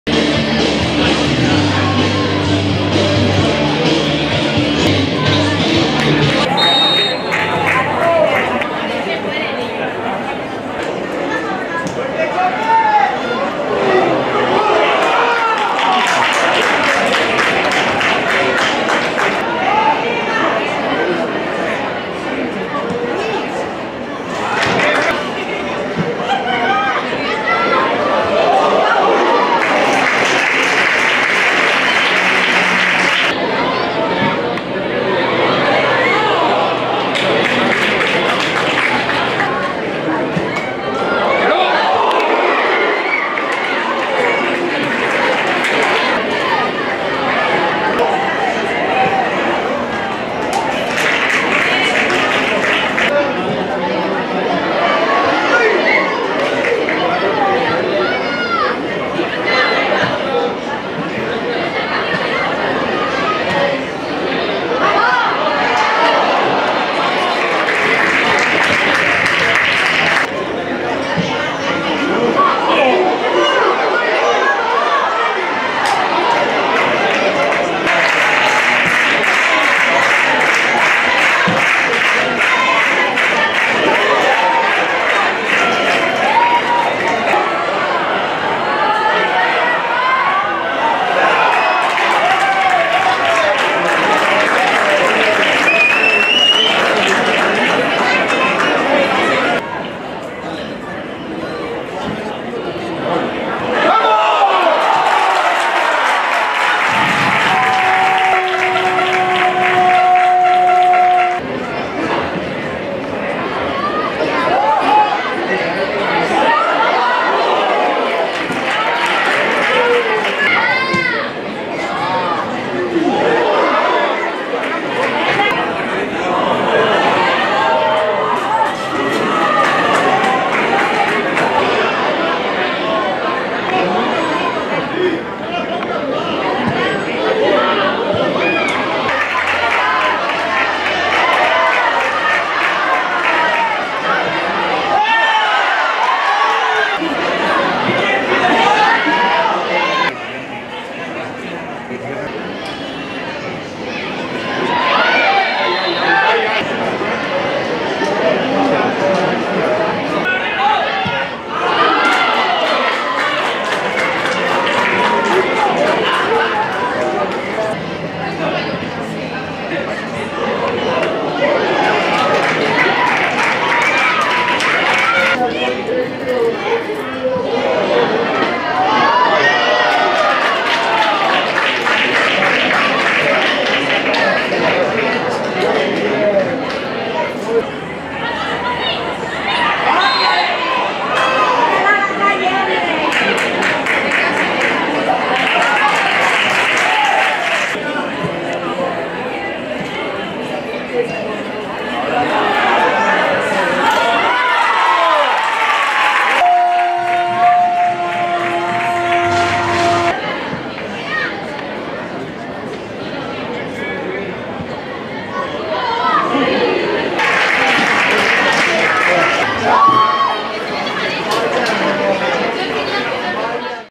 Resum Femení 2 - Fundación Albacete 4
El Femení, superat per un Albacete molt efectiu

El Femenino, superado por un Albacete muy efectivo